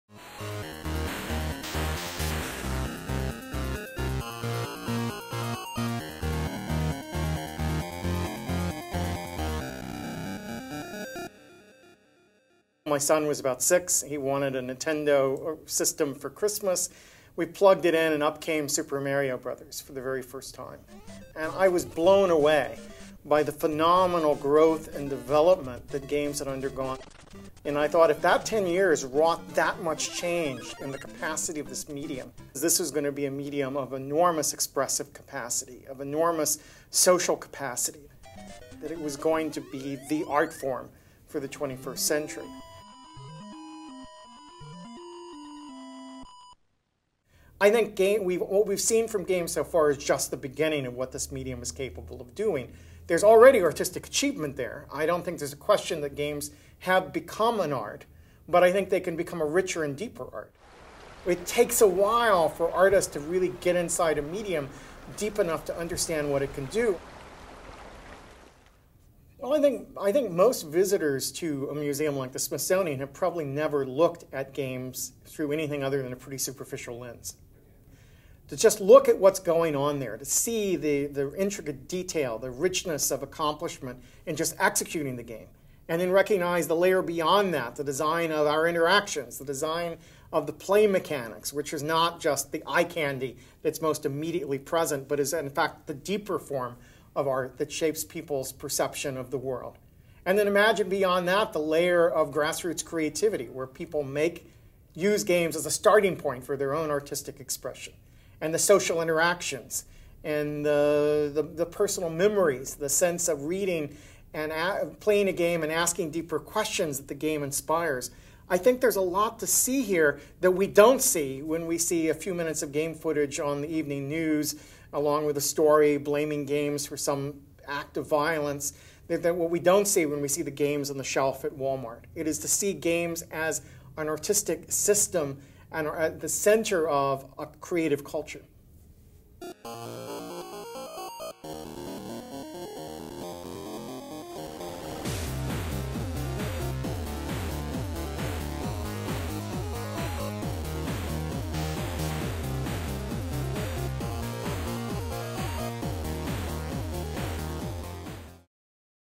The Art of Video Games: Interview with Henry Jenkins
The Art of Video Games exhibition was on view at the Smithsonian American Art Museum March 16 through September 30, 2012.